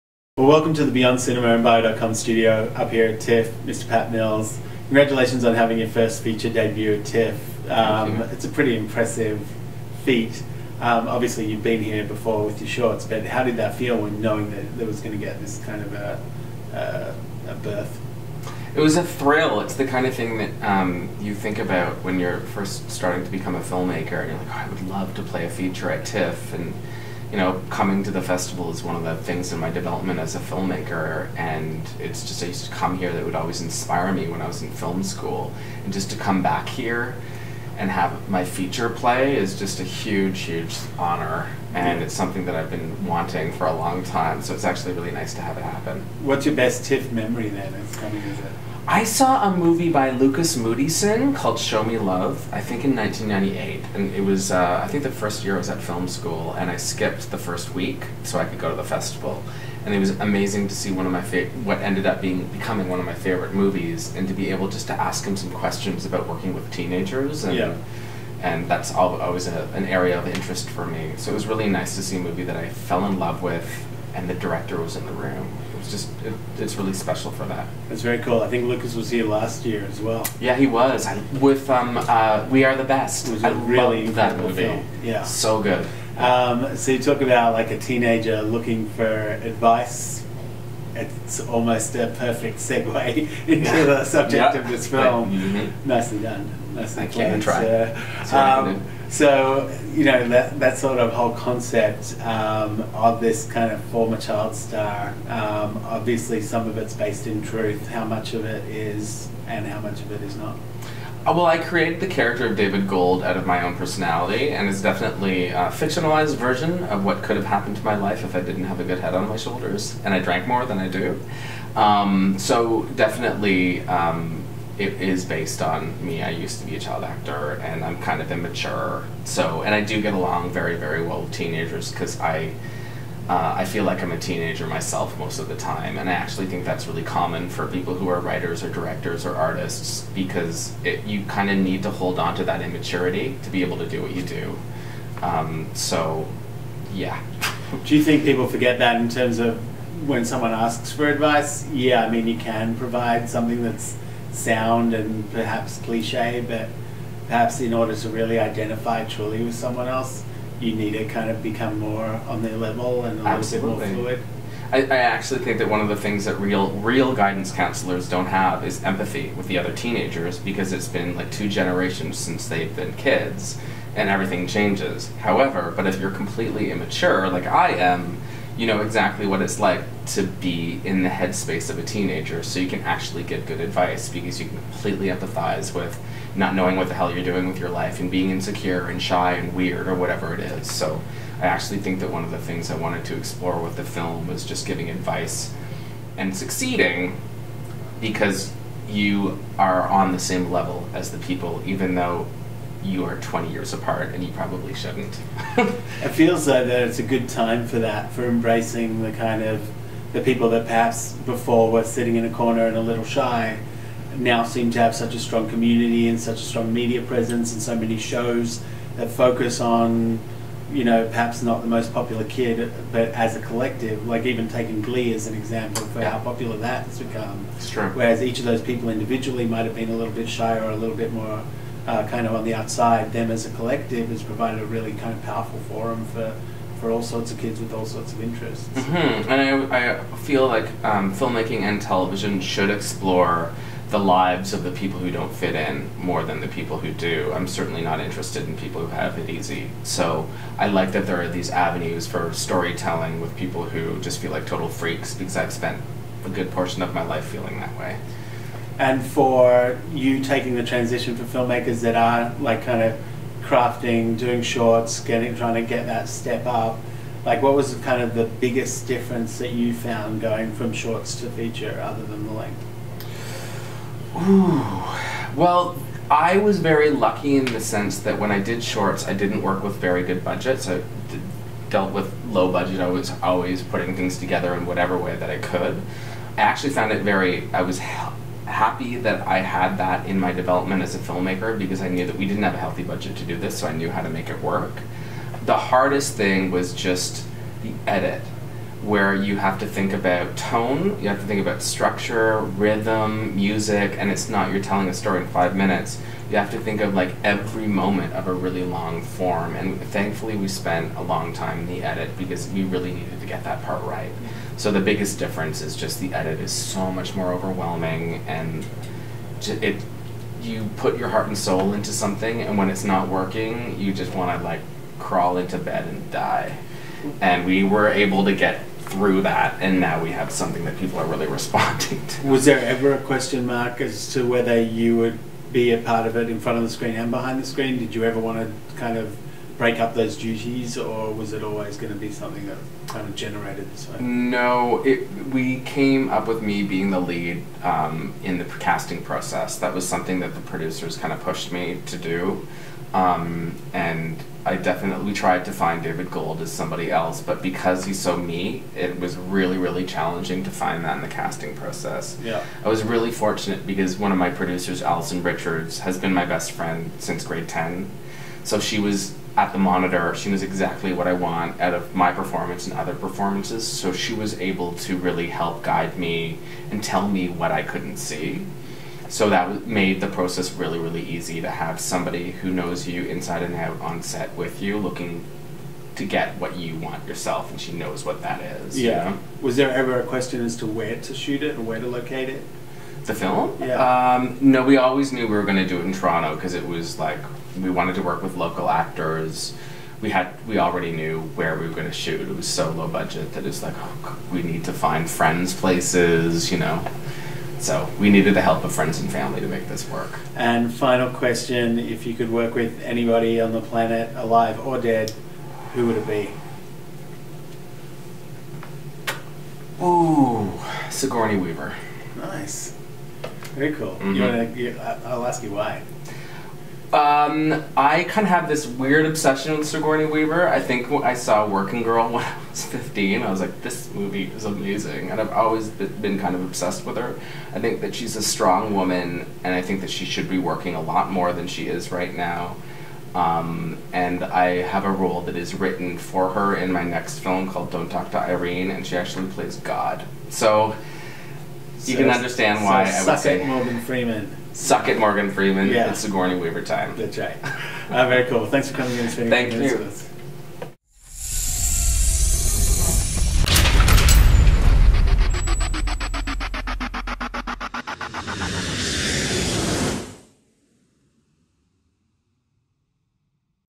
Pat Mills talks GUIDANCE at the Beyond Cinema & Bio.com TIFF Studio
Elliot V. Kotek interviews Pat Mills about his new film "Guidance" at the Beyond Cinema & Bio.com Studio at the 2014 Toronto Film Festival.